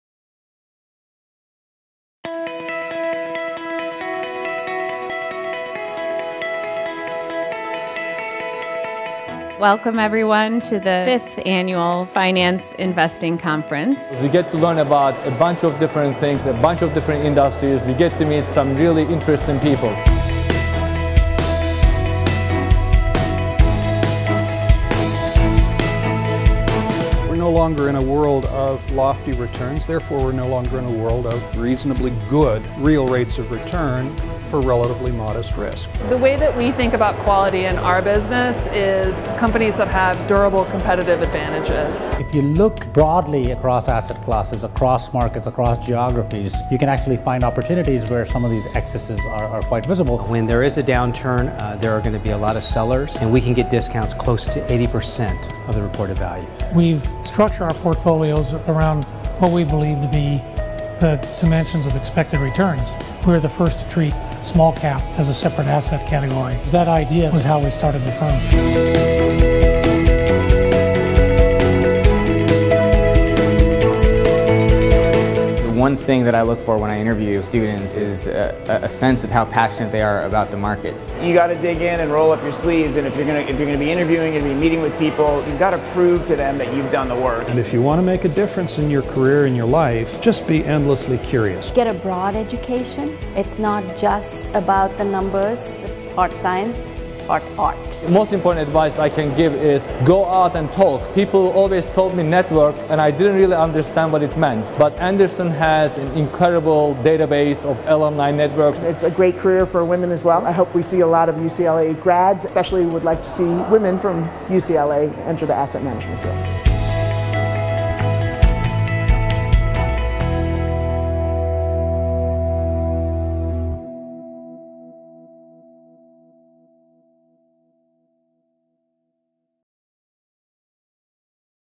What Makes UCLA Anderson's Finance Investing Conference So Valuable?
UCLA Anderson's 5th annual Finance Investing Conference brought students face to face with some of the brightest minds in the biz. Here, they share their thoughts on the conference - and how it helps students in pursuit of a finance-based career.